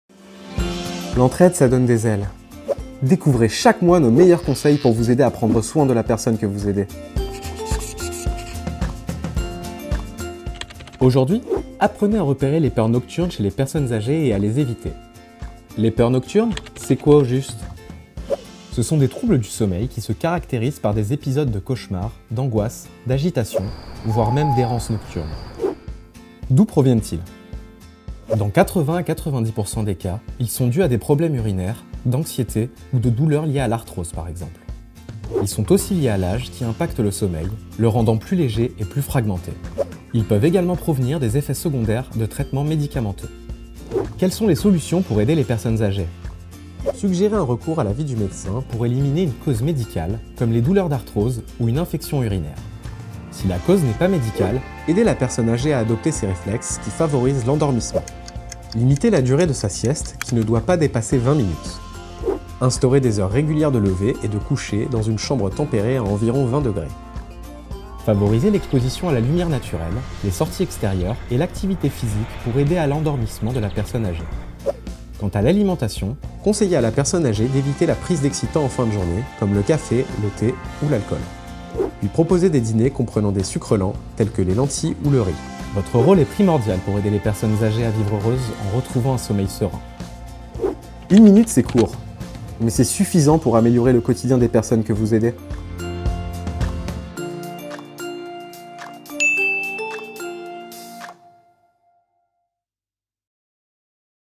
Une minute pour aider - Les peurs nocturnes
Pour plus d’infos sur le métier d'auxiliaire de vie avec Petits-fils

Découvrez comment se caractérisent les peurs nocturnes chez une personne âgée, ainsi que les moyens d'y faire face.